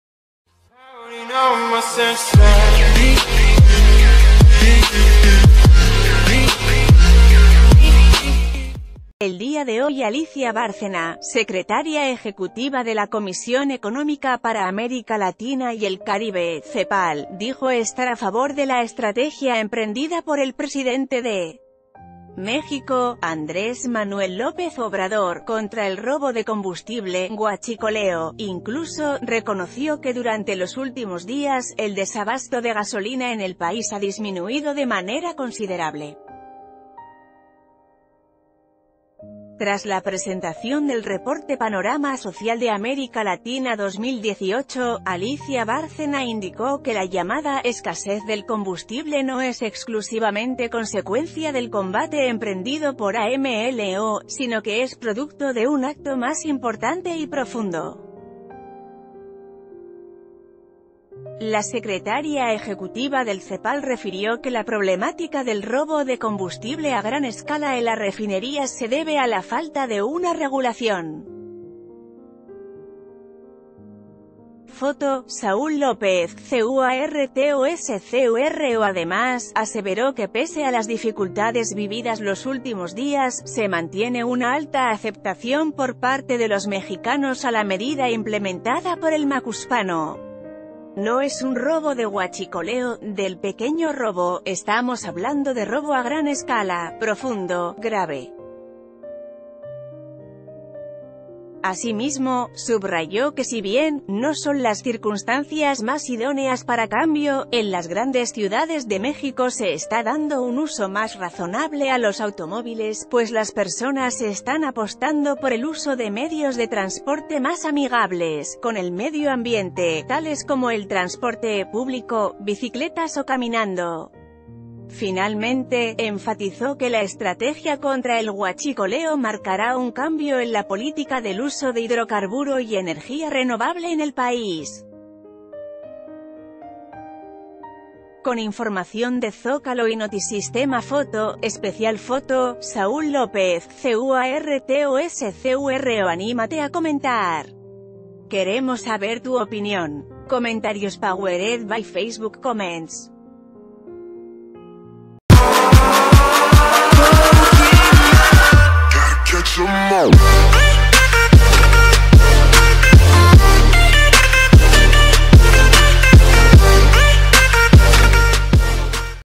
Cepal aprueba y apoya estrategia de AMLO contra el huachicoleo
El día de hoy Alicia Bárcena, secretaria ejecutiva de la Comisión Económica para América Latina y el Caribe (Cepal), dijo estar a favor de la estrategia emprendida por el presidente de México, Andrés Manuel López Obrador, contra el robo de combustible (huachicoleo); incluso, reconoció que durante lo...